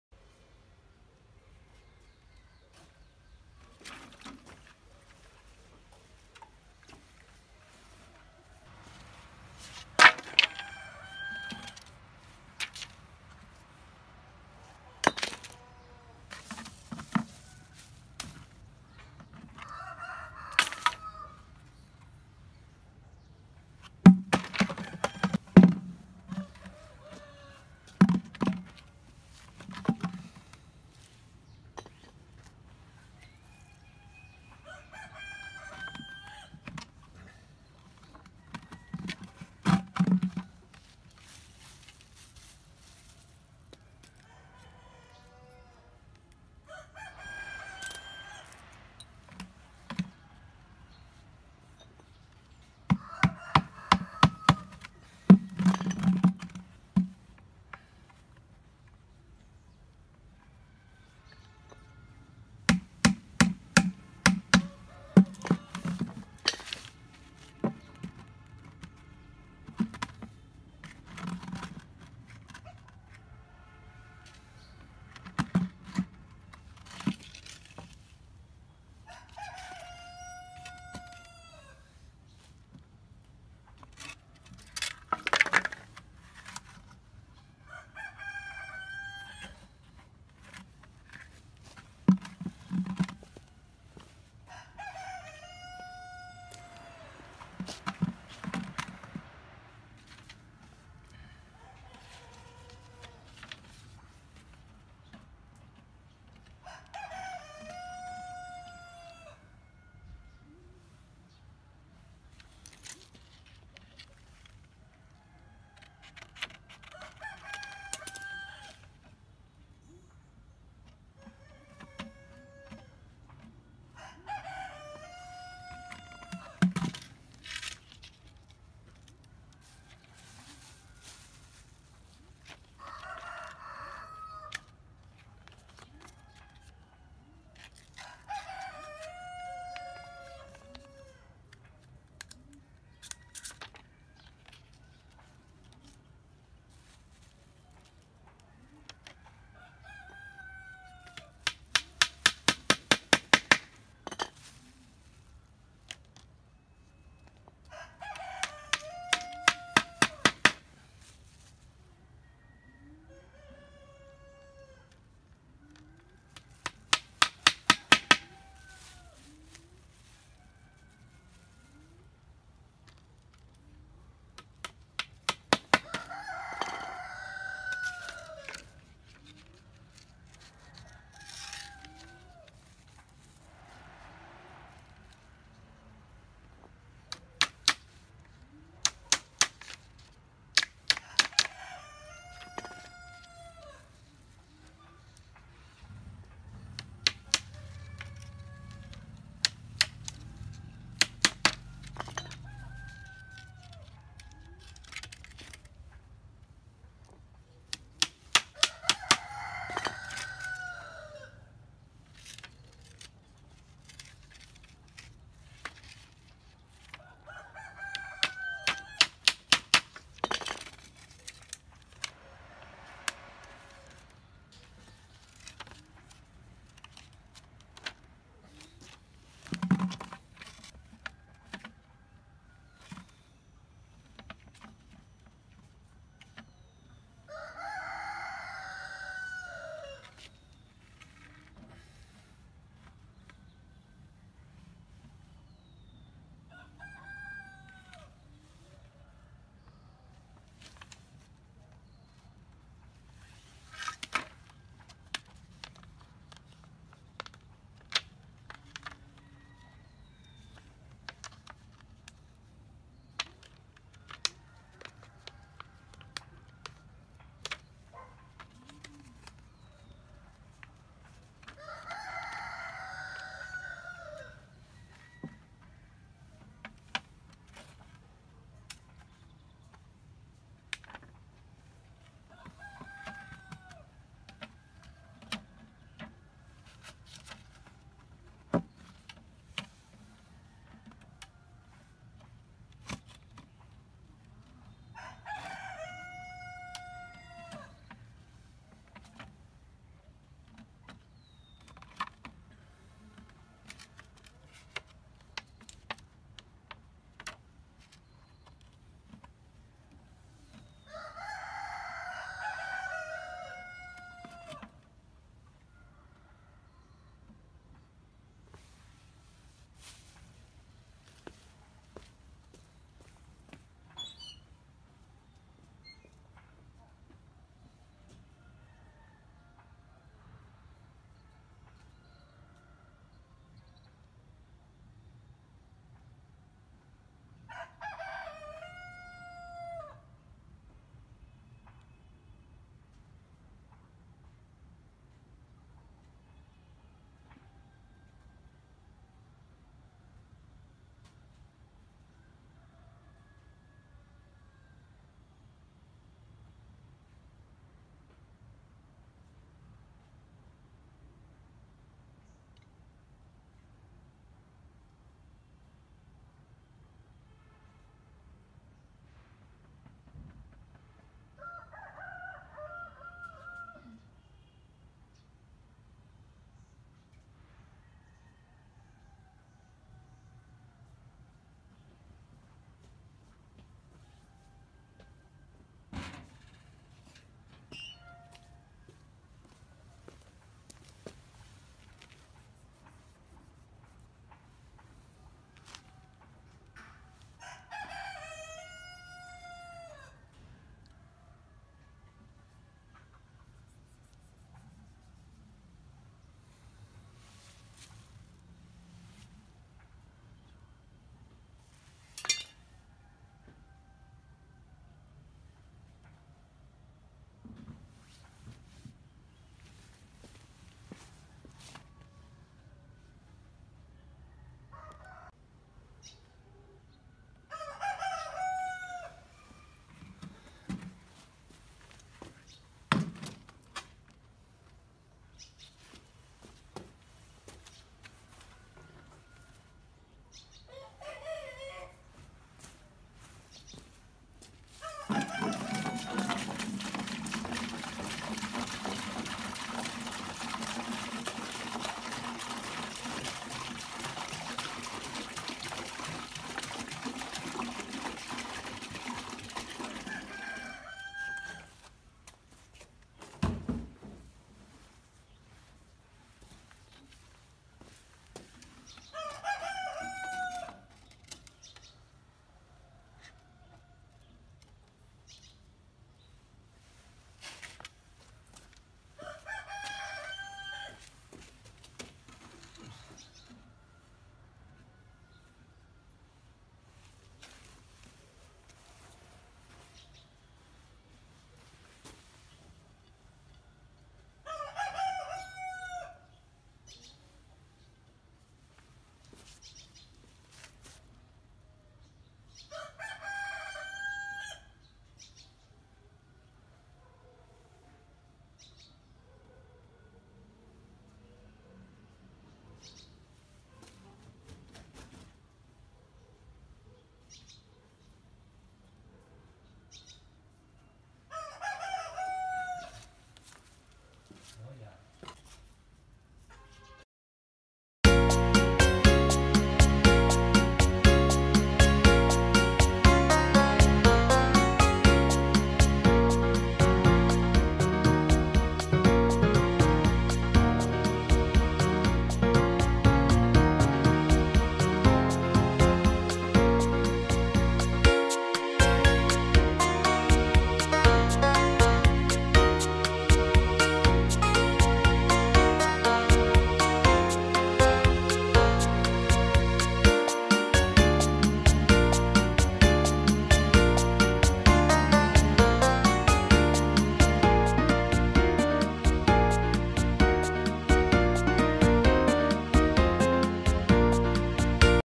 Trampa para ratones / Trampa para ratas / Trampa estúpida Guardar un ratón estúpido
Trampa estúpida que salva a un ratón estúpido. Cómo hacer una trampa para ratas con tubería de agua de PVC ...